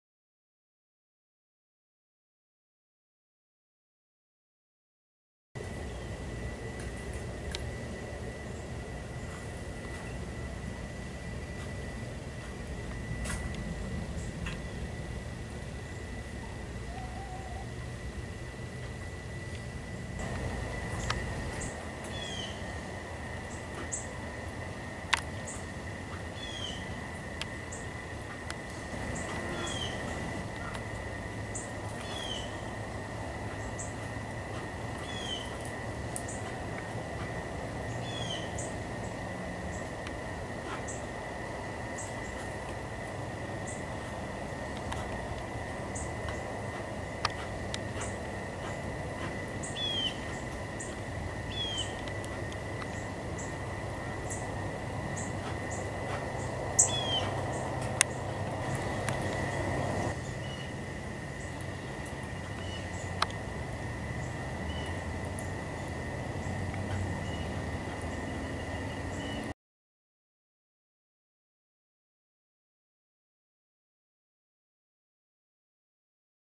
Grey tree squirrel with bot fly
This seems real bad but hopefully the squirrel will recover from this,but it is sad to watch a healthy animal drained of life.I myself don't mined a squirrel at the feeder and enjoy seeing a new squirrel at the feeders and watching him grow to become a nice healthy squirrel,so seeing this is sad but they do recover and as long as they have food and water they have the the best chance at recovery.